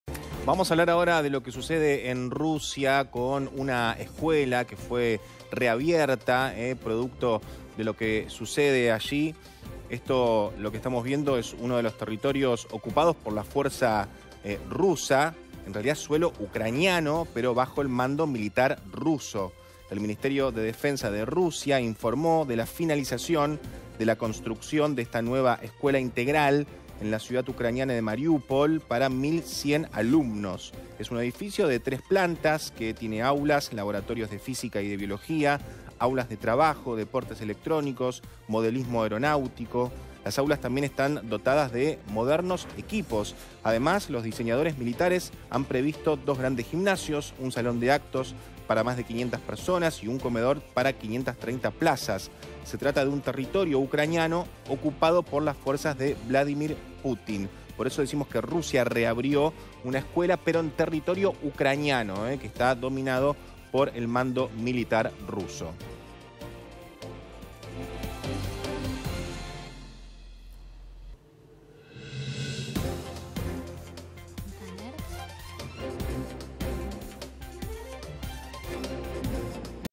Rusia reabrió escuela en Mariúpol
👉 El Ministerio de Defensa de Rusia informó de la finalización de la construcción de una nueva escuela integral en la ciudad ucraniana de Mariúpol para 1.100 alumno.